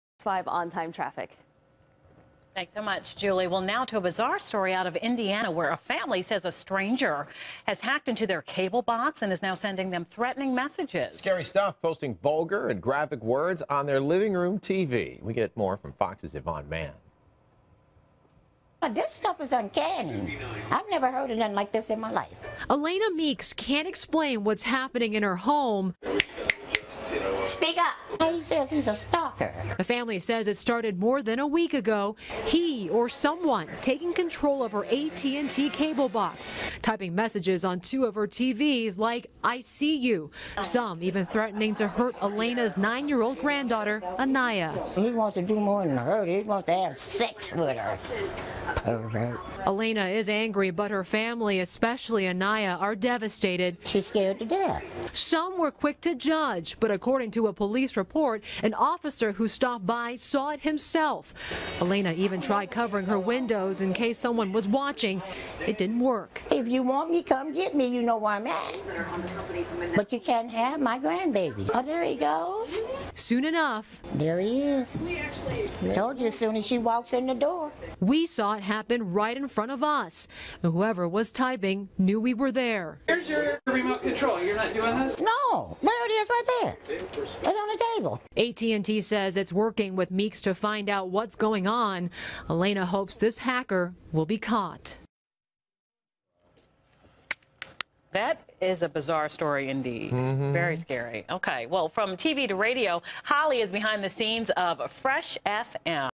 Family Terrorized By Cable Box
You read that right.  A hacker has taken control of a woman s cable box and has been threatening the family. WTF. -